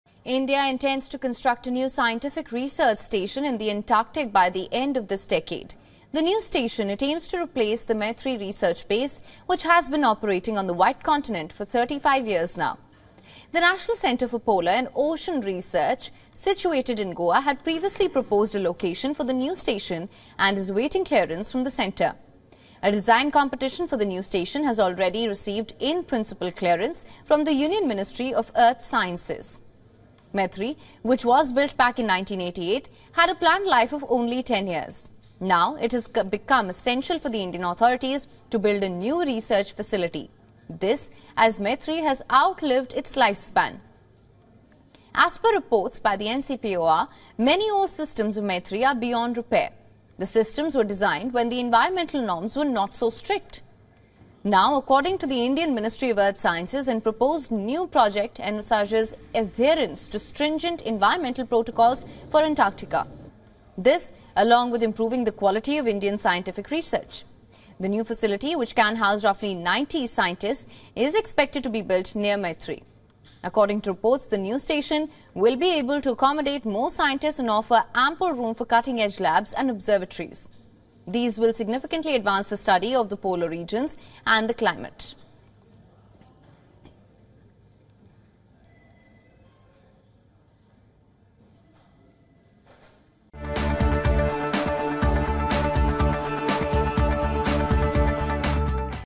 India to construct new scientific research station in Antarctic by Jan 2030 | Latest News | WION
India's Antarctic research base, 'Maitri,' built in 1988 and had a lifespan of just ten years. However, the Antarctic base has outlived its original lifespan. According to reports, Maitri is now beyond repairs.